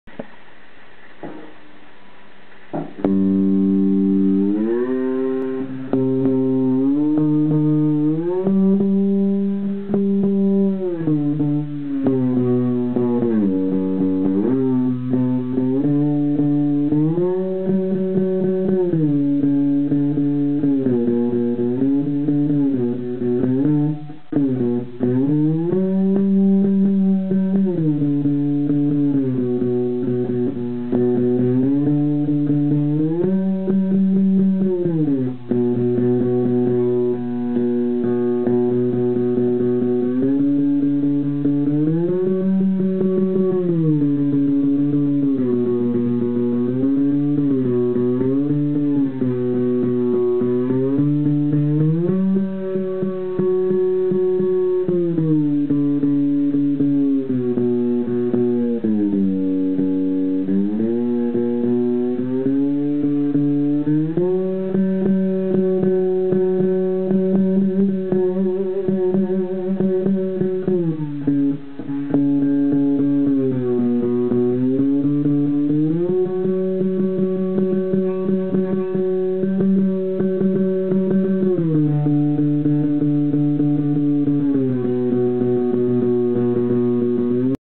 Pool Cue Diddley Bow - Having a go.
Well, is is snowing outside, 5th Feb, and it is the wifes birthday today, so PERFECT.
I had a go at this stick again, and thought I would upload my half arsed, tone deaf attempt.
The more I play with it, the less the cats run away. am I wearing them down ?